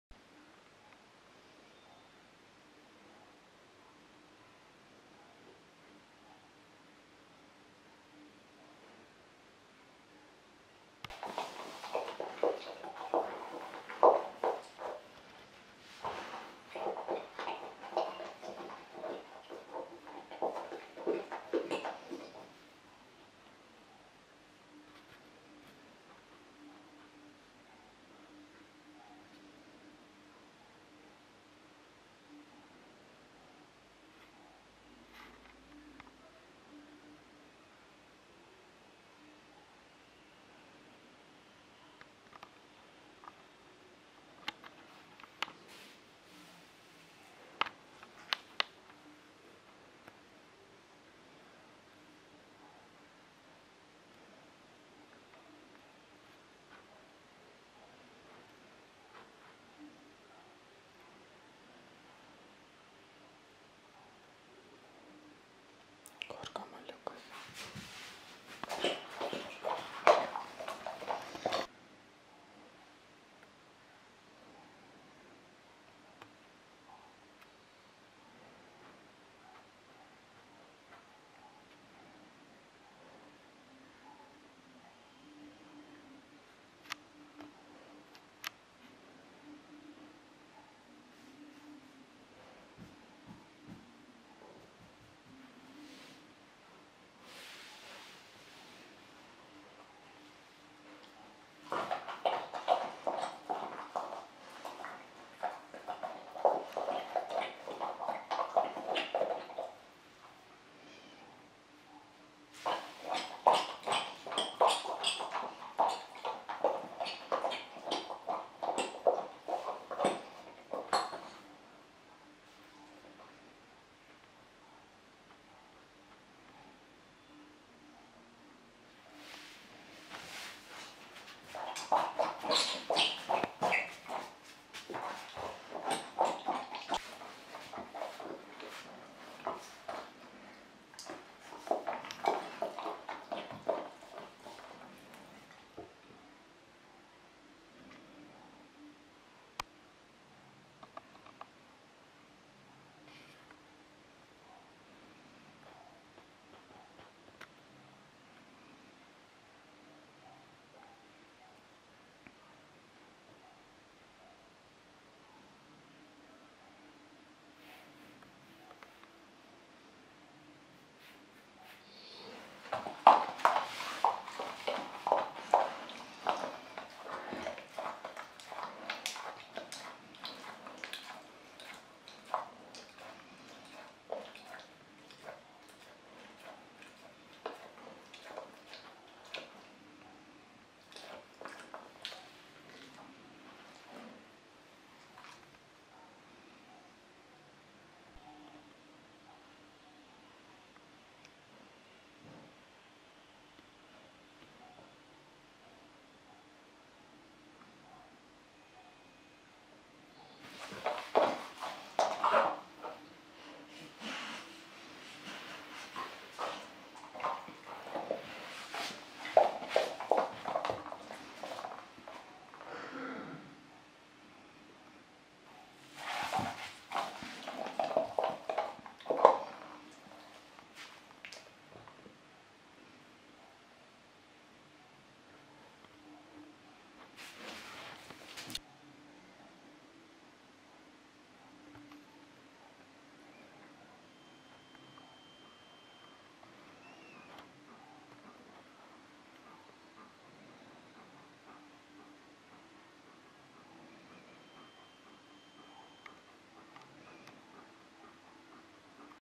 Nikon D5600 Video test #5 Belgian Malinois in Dark environment
Nikon D5600
AF-P Nikkor 18-55mm 3.5-5.6G VR
1920x1080 24p
1/30 shutter speed
This is my Belgian Malinois.